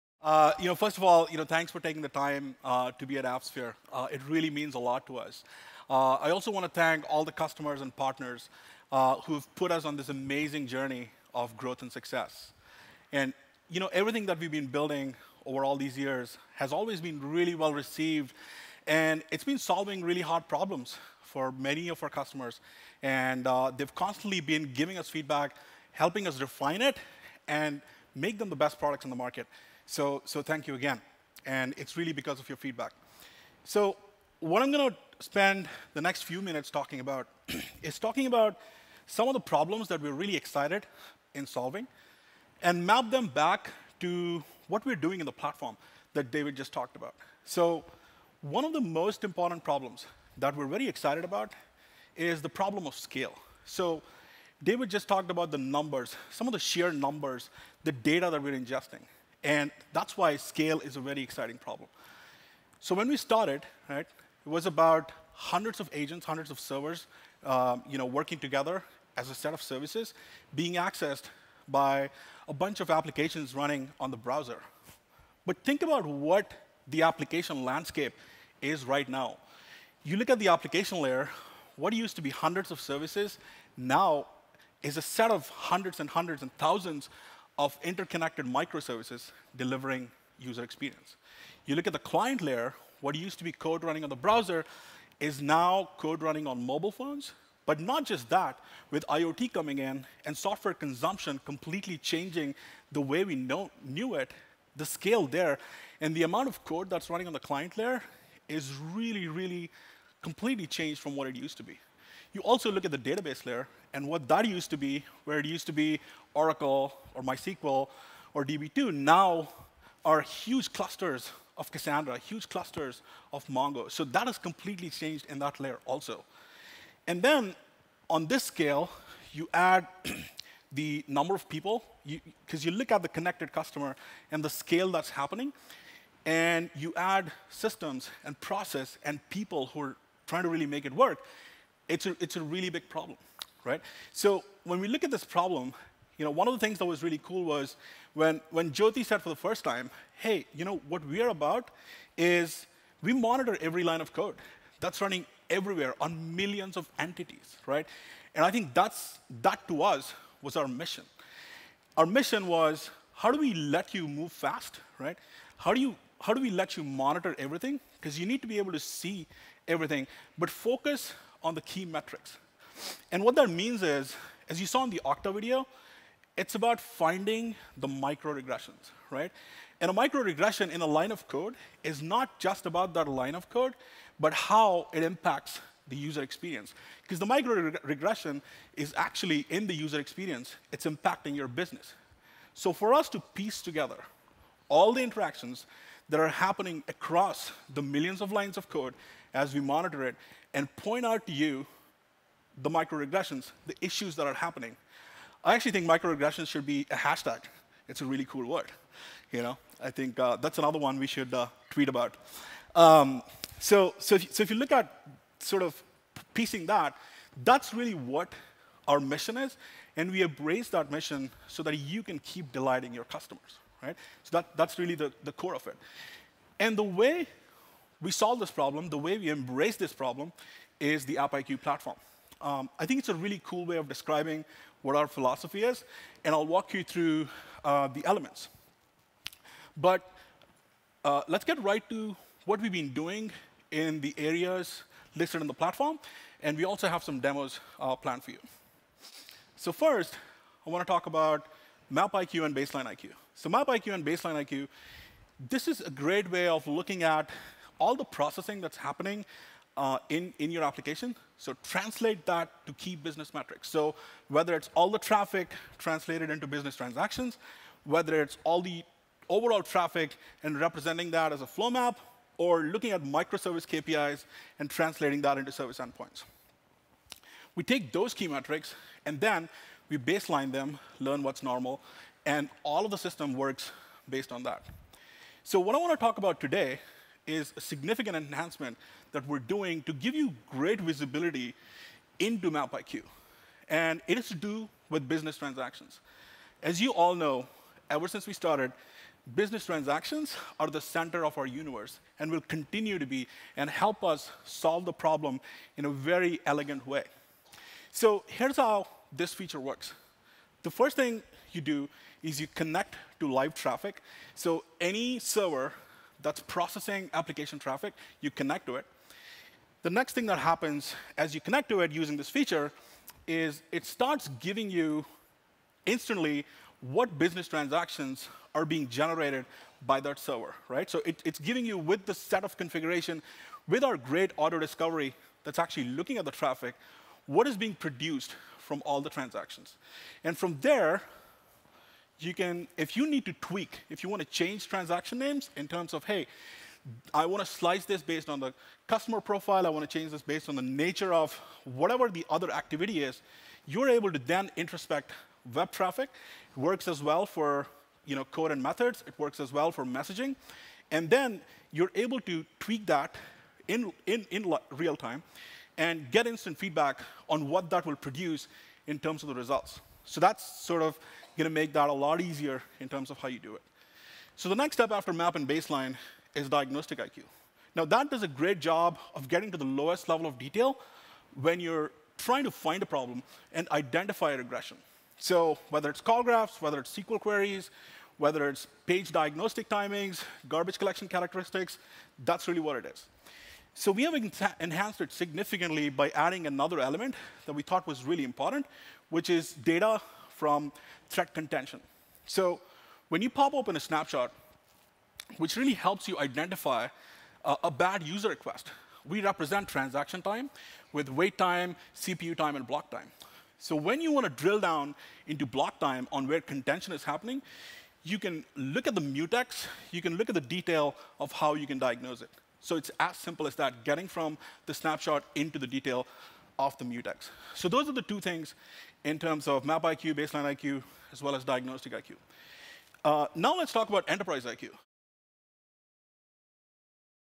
How to Scale Application Performance Management with AppDynamics App iQ
Bhaskar Sunkara, CTO of AppDynamics, describes the App iQ platform and how App iQ addresses massive enterprise scale deployments. Pinpoint micro-regressions quickly and understand how each code deployment affects customer experiences.

AppSphere16 Day 1 Keynote | Chapter 4 of 14